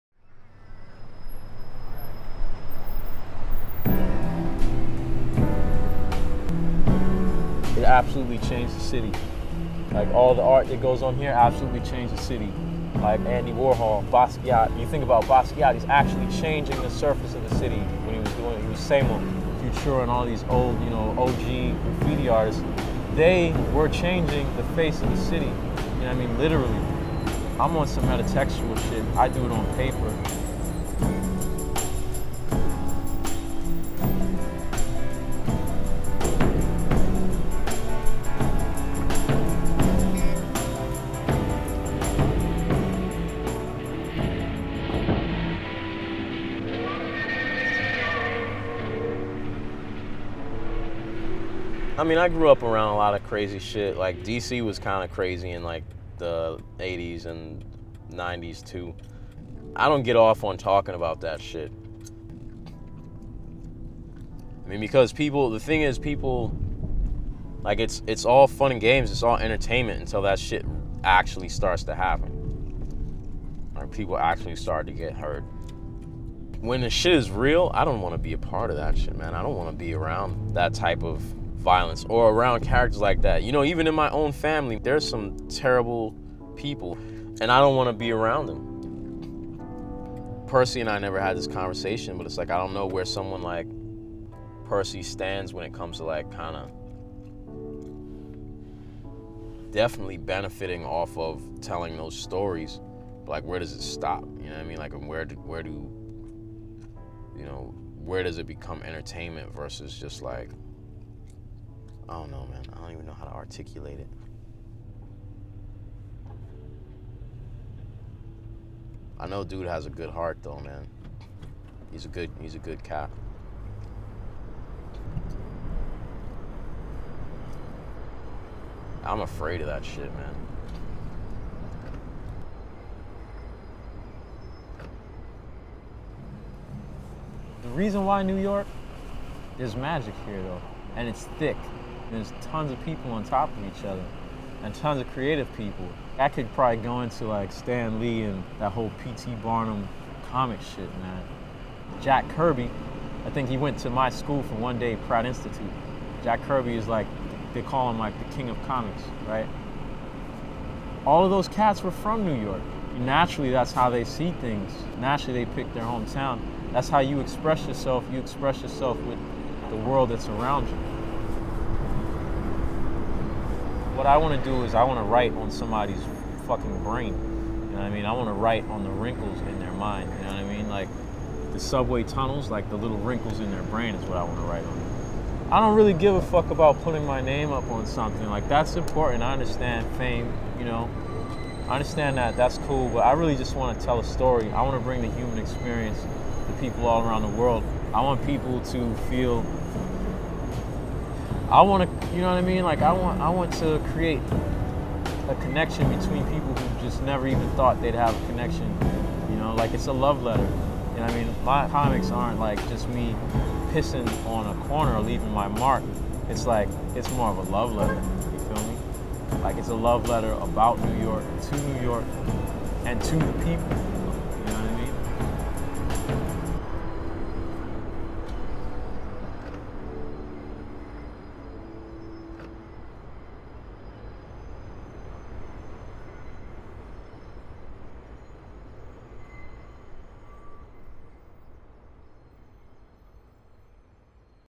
Hip Hop Jack Kirby
RonWimberly gots super-powers.
The Brooklyn-based artist is one of the coolest creators of our generation.
Nike hired him to produce the "Calvin & Johnson" cartoon, starring Detroit Lions All-Pro and Diddy.
He also signed "Sentences" a cult comics about the life of Rapper MF Grimm.
Here, Ron opens up frankly about super-powers and super-shitty situations.